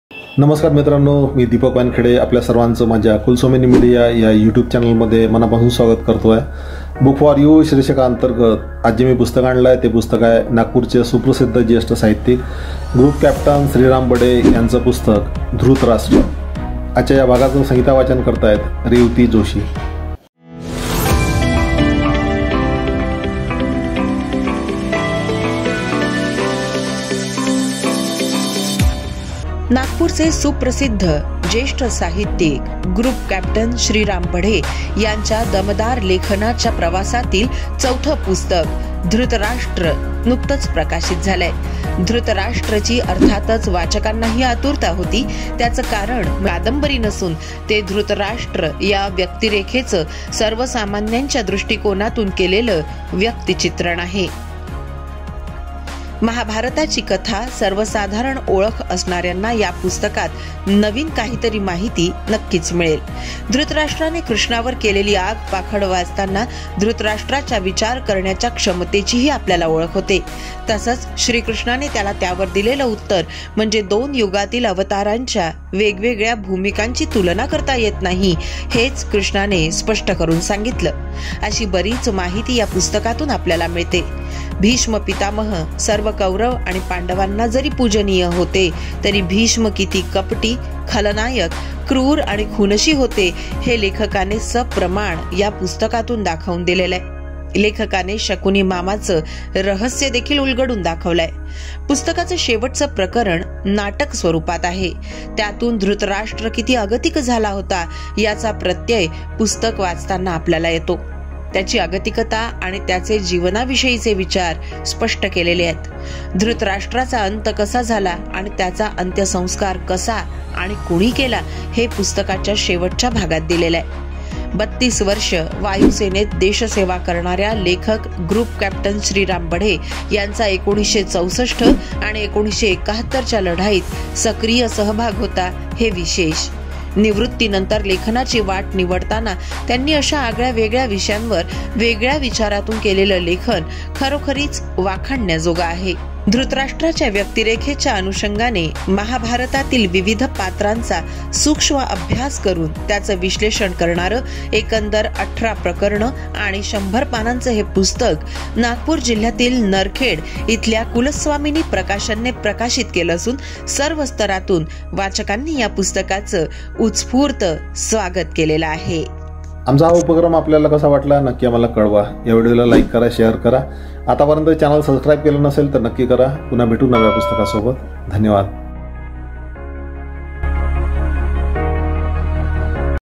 धृतराष्ट्र /Marathi Book Review/ लेखक - ग्रुपकॅप्टन श्रीराम बढे
Marathi Book Review  No.- 30
पुस्तक - धृतराष्ट्र
लेखक - ग्रुपकॅप्टन श्रीराम बढे
Hello everyone.
You know, about my activity, Marathi Hindi and English book reviews.
Today i chose Marathi book -
पुस्तक - धृतराष्ट्र
प्रकाशक :कुलस्वामिनी प्रकाशन नरखेड, जि नागपूर
 किंमत: 150/-
Author and publisher can send their book to me for reviews on my address, if they wants...
 - DEEPAK WANKHEDE